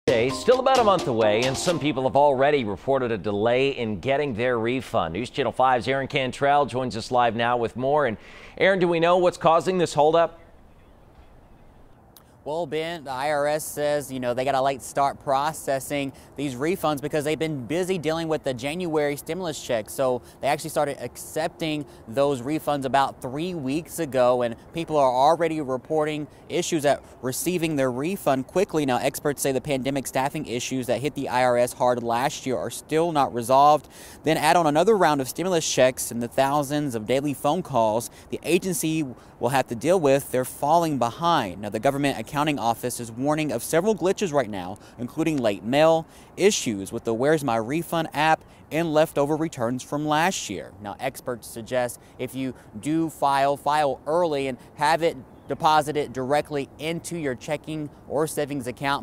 Tax refunds delayed; what you need to know
Experts suggest people file electronically, request direct deposit and file sooner rather than later.